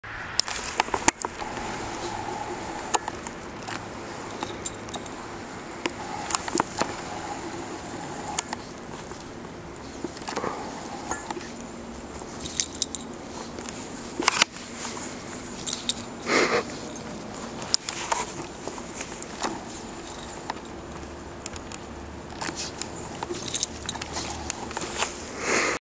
american bulldog pups playing in snow
sweetie and magnum playing in snow